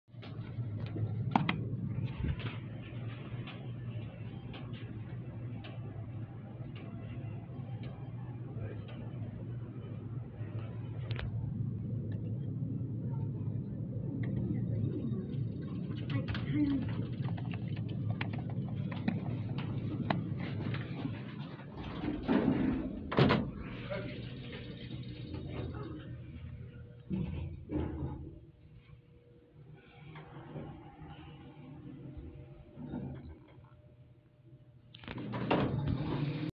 三菱エレベーター
大歩危峡まんなかにあります。まだ未更新のままで運転しています。機種は中期エレペットアドバンスvです。
メーカー:三菱
設置年:1987年
定員:9名
積載:600kg
キャンセル機能:なし
アナウンス:なし
戸閉ブザー:なし
到着ベル音:なし
ボタン確認音:なし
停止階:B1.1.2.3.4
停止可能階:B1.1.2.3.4
管理会社:SECエレベーター